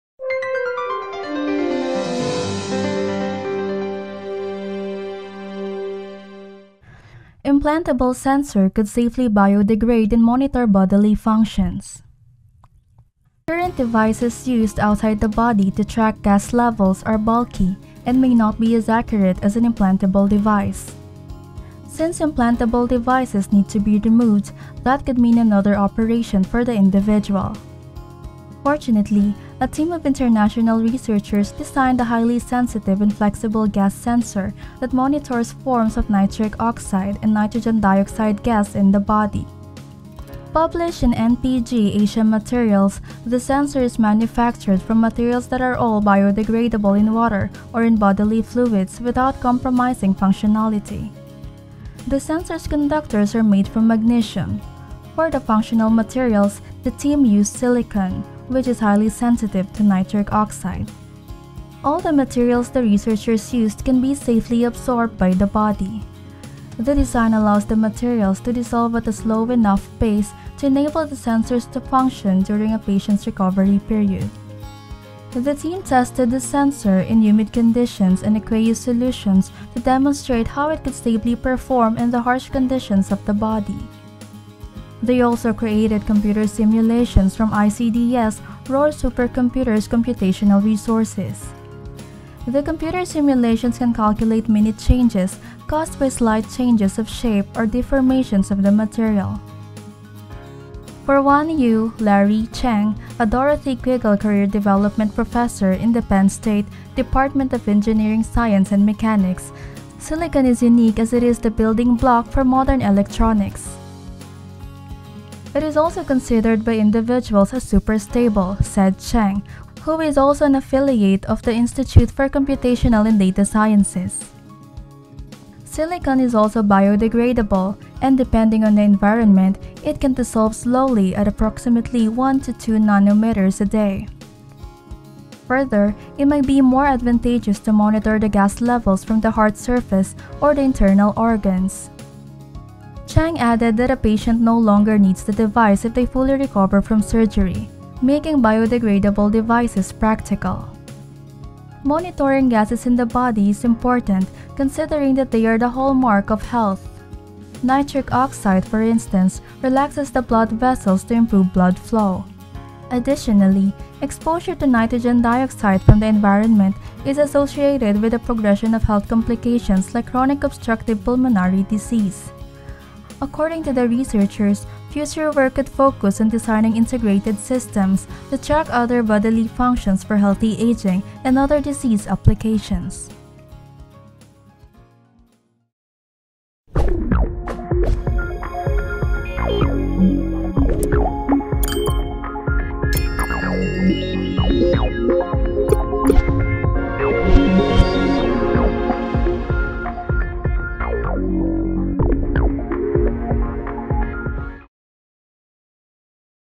Implantable Sensor Could Safely Biodegrade and Monitor Bodily Functions
A team of researchers designed an implantable sensor that could safely biodegrade and monitor bodily functions. They used magnesium and silicon to create the device.